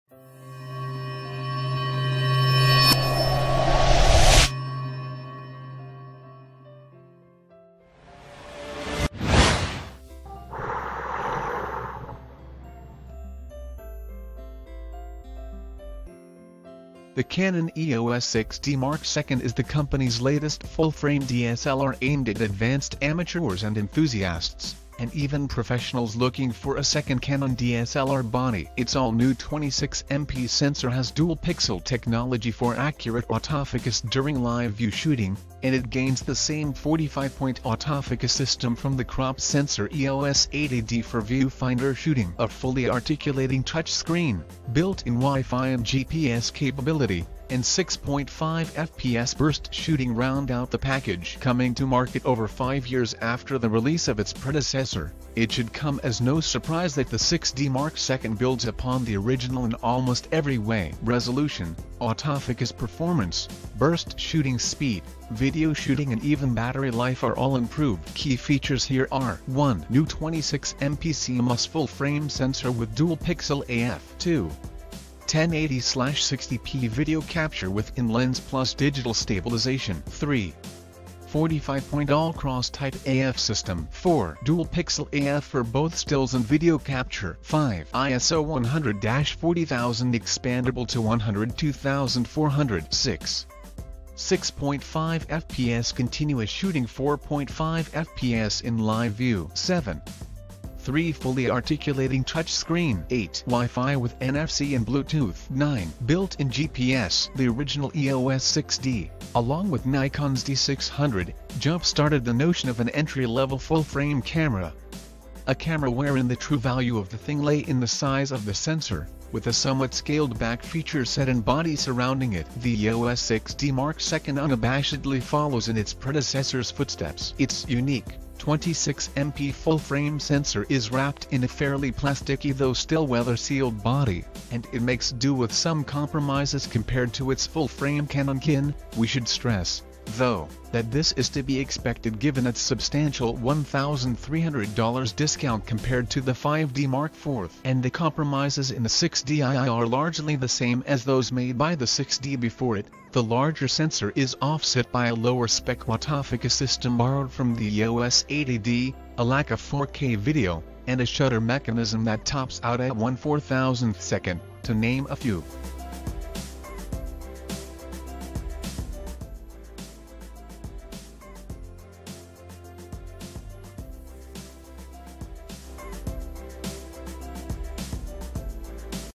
Canon EOS 6D Mark II Key Features and Specs
The Canon EOS 6D Mark II is the company's latest full-frame DSLR aimed at advanced amateurs and enthusiasts, and even professionals looking for a second Canon DSLR body. Its all-new 26MP sensor has Dual Pixel technology for accurate autofocus during live view shooting, and it gains the same 45-point autofocus system from the crop-sensor EOS 80D for viewfinder shooting. A fully articulating touchscreen, built-in Wi-Fi and GPS capability, and 6.5 fps burst shooting round out the package.